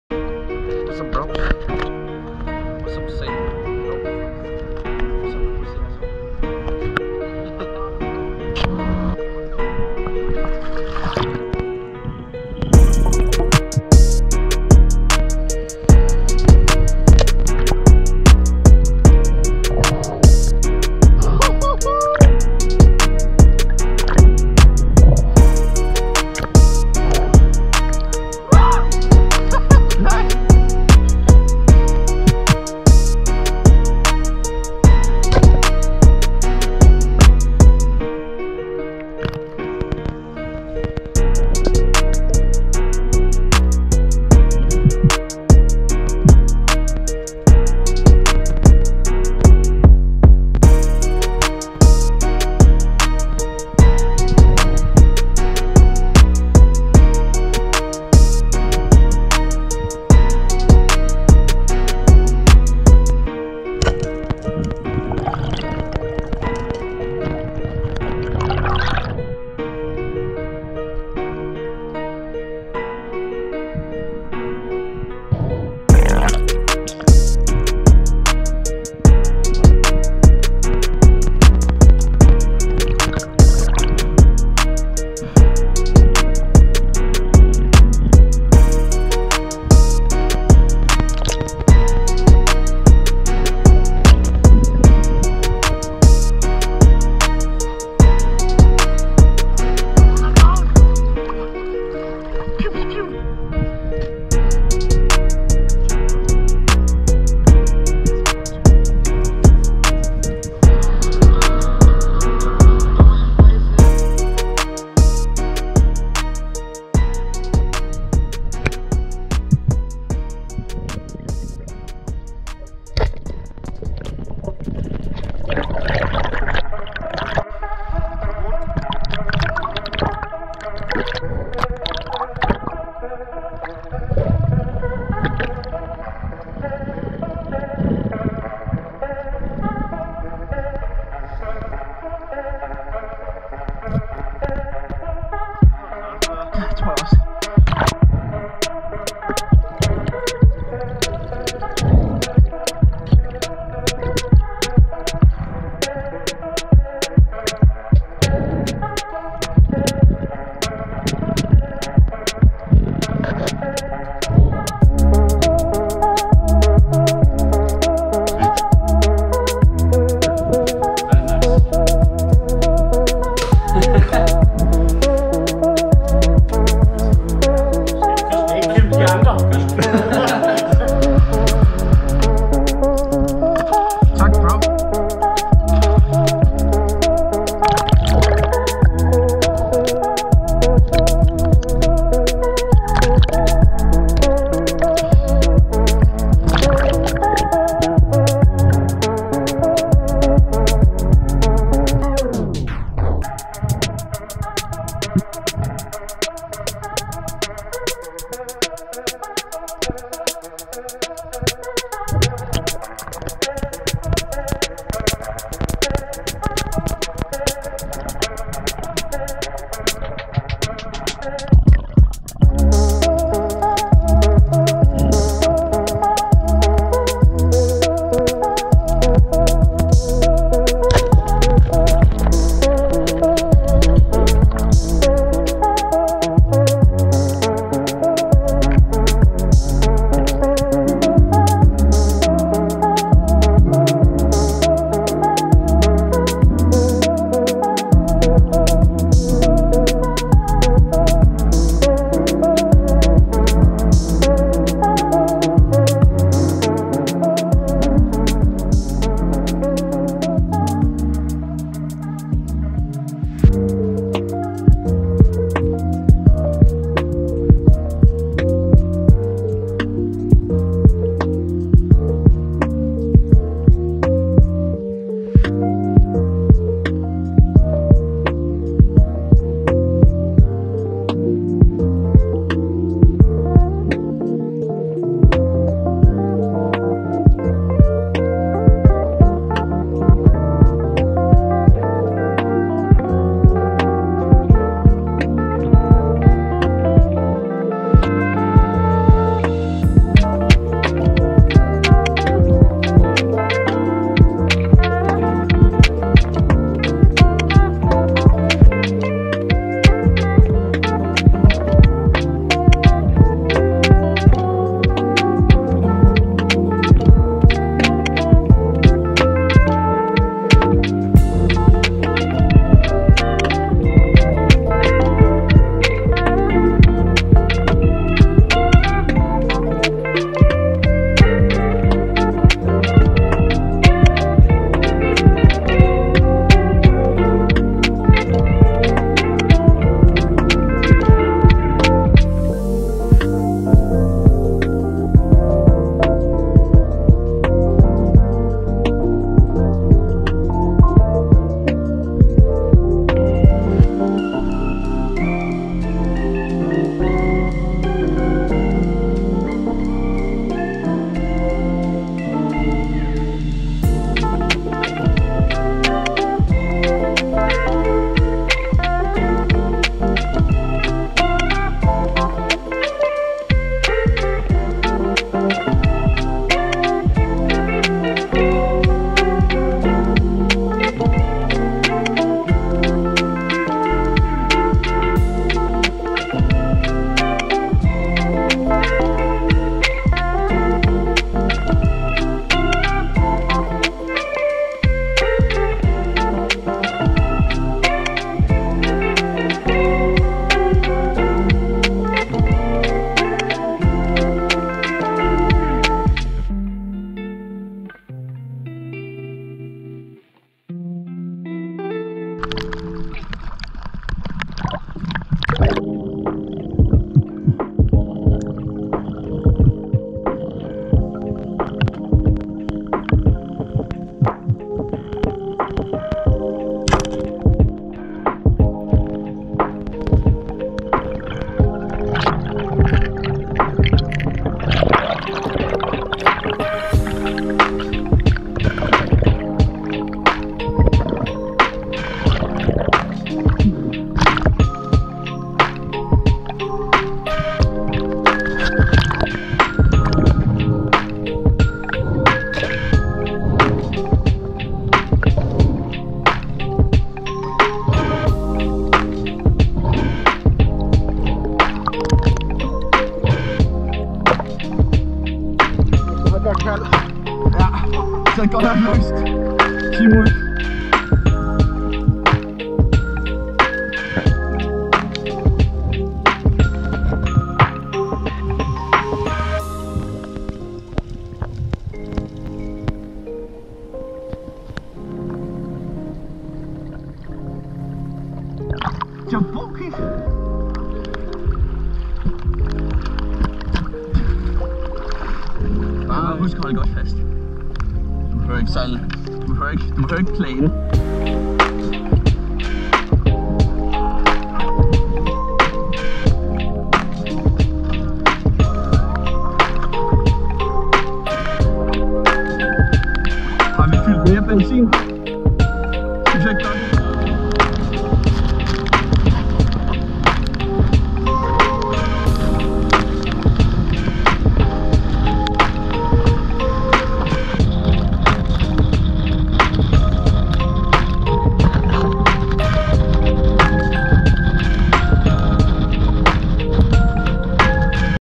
Spearfishing Norway 2020
Spearfishing with an amazing group of friends in Norway summer of 2020.

UV-jagt i Norge sommeren 2020 med en gruppe fantastiske venner. Torsk, lubbe, havtaske, taskekrabber og kammuslinger.